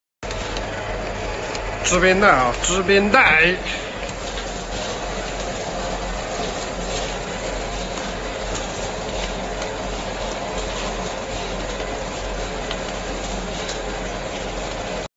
satin label lanyard tape heat transfer machine sublimation calendar
Our lanyard heat transfer machine also can print is satin label, see as below video...
YS-200600Z Heat Press
Heating power - 5 kw
Diameter of oil drum - 200 mm/7.8″
Printing speed - 50-70m/hour/piece, can put 10-30pcs/time
Transfer width - 600mm/23.6″
Max temperature - 240℃
Function single side and double side printing - Yes
Application: drawstring, ribbon, railing belts, hair belts, waist belts, restraint belts, elastic belts, shoe laces, shoulder straps, gift belts, pet belts, etc.